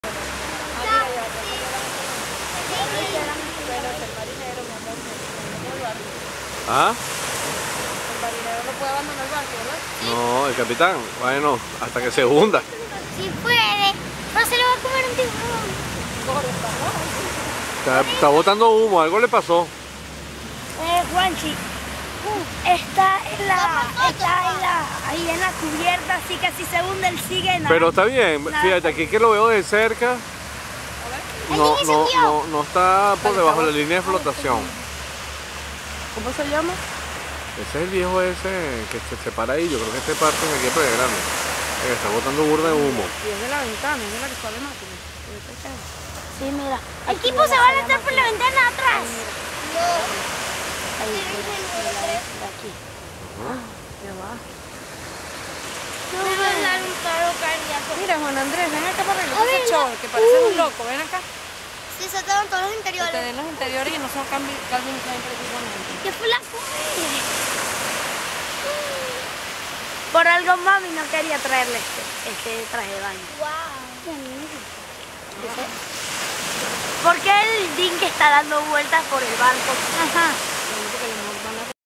SAFLOMAR II Hundiendoce Playa Grande 18-2-2012 Video1.MOV
Yate SAFLOMAR II Hundiendoce frente a Playa Grande Yachting Club el dia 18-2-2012 entre 6:30 a 7am. Video1.